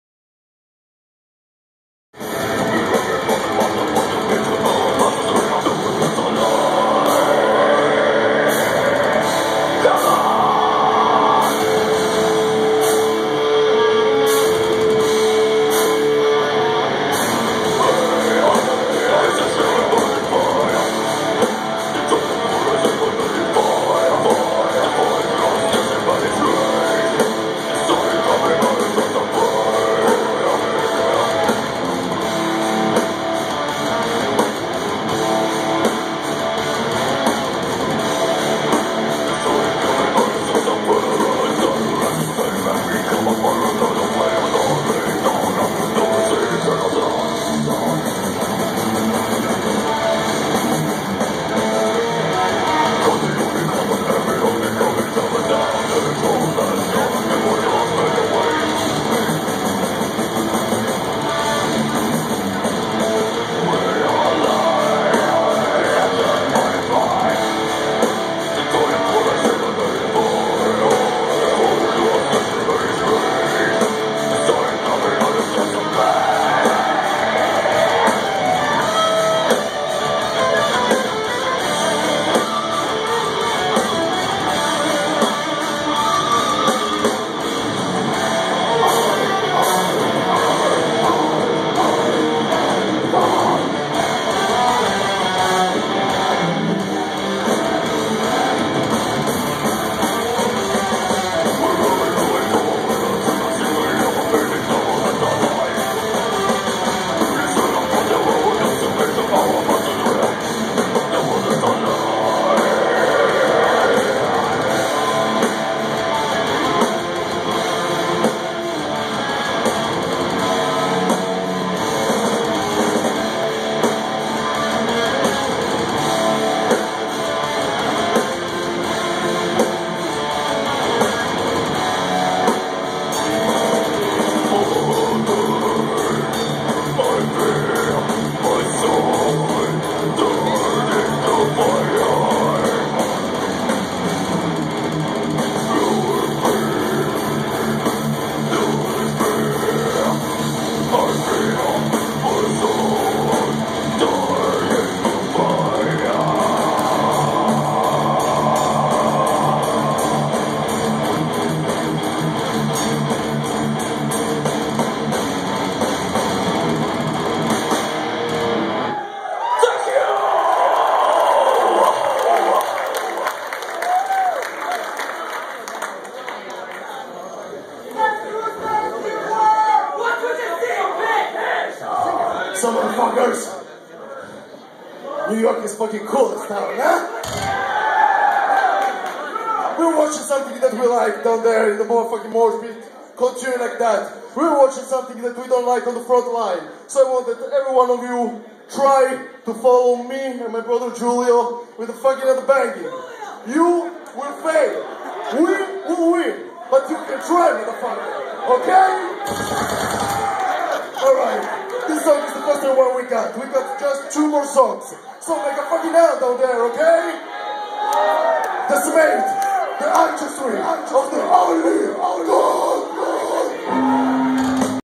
Hour of Pennance Vid 2 | Irving Plaza NYC | 4/19/2012
Italian Death Metal. The lighting made the video come out dark.